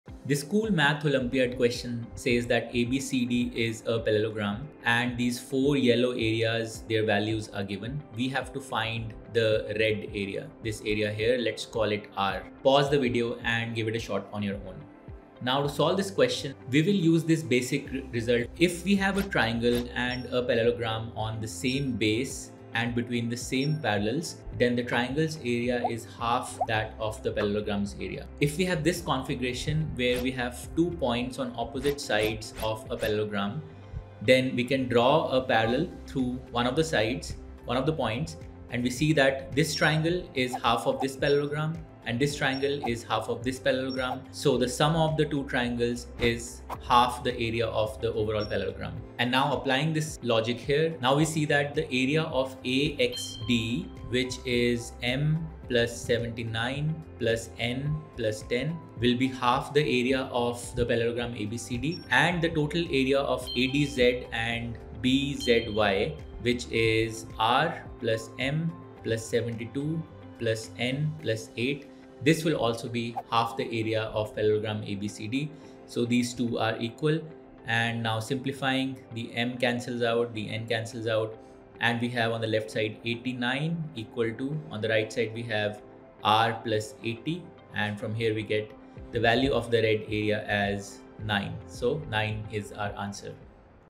This was asked in a Chinese Grade 5 exam! 🤯 Parallelogram and Triangles Problem - Math Olympiad Prep
This beautiful problem uses the simple geometry result:
"Area of the triangle is half the area of the parallelogram with the same base and height."

Apparently it was asked in one of the Chinese exams meant of 5th grade gifted students.